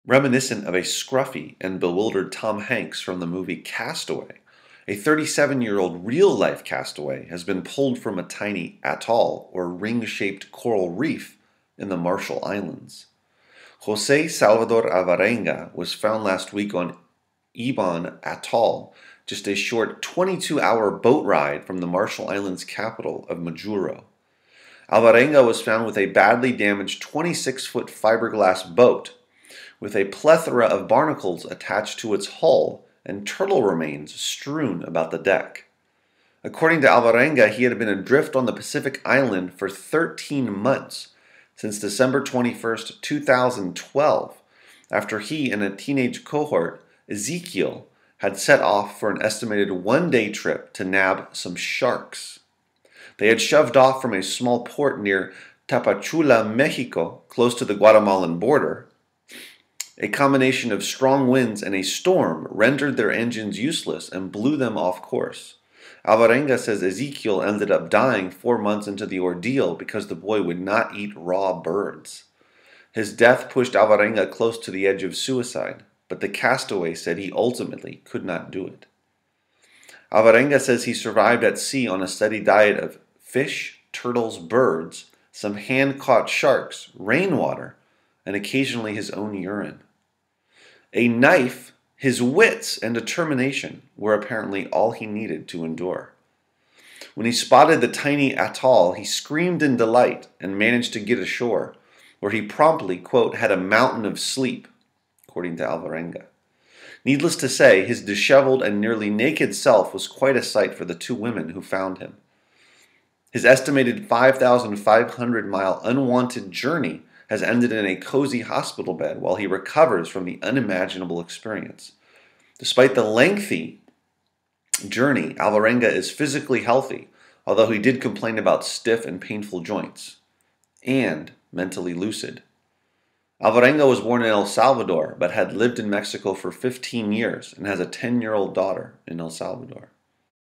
El Salvadorean Castaway Claims He Spent 13 Months in Pacific Ocean
Reminiscent of a scruffy and bewildered Tom Hanks from the movie "Castaway," a 37-year-old real life castaway has been pulled from a tiny atoll (At-All), or ring-shaped coral reef, in the Marshall Islands. Jose Salvador Alvarenga was found last week on Ebon Atoll, just a short 22-hour boat ride from the Marshall Islands capital of Majuro. Alvarenga was found with a badly damaged 26-foot fiberglass boat with a plethora of barnacles attached to its hull and turtle remains strewn about the deck.

According to Alvarenga he had been adrift on the Pacific Island for 13 months, since December 21, 2012 after he and a teenage cohort, Ezekiel, had set off for an estimated one-day trip to nab some sharks. They had shoved off from a small port near Tapachula, Mexico close to the Guatemalan border. A combination of strong winds and a storm rendered their engines useless and blew them off course. Alvarenga says Ezekiel ended up dying four months into the ordeal because the boy would not eat raw birds. His death pushed Alvarenga close to the edge of suicide, but the castaway said he ultimately could not do it.

Alvarenga says he survived at sea on a steady diet of fish, turtles, birds, some hand caught sharks rainwater and occasionally his own urine. A knife, his wits and determination were apparently all he needed to endure. When he spotted the tiny atoll he screamed in delight and managed to get ashore, where he promptly "had a mountain of sleep" according to Alvarenga. Needless to say, his disheveled and nearly naked self was quite a sight for the two women who found him.

His estimated 5,500 mile unwanted journey has ended in a cozy hospital bed while he recovers from the unimaginable experience. Despite the lengthy experience Alvarenga is physically healthy, although he did complain about stiff and painful joints, and mentally lucid. Alvarenga was born in El Salvador but had lived in Mexico for 15 years and has a 10-year-old daughter in El Salvador.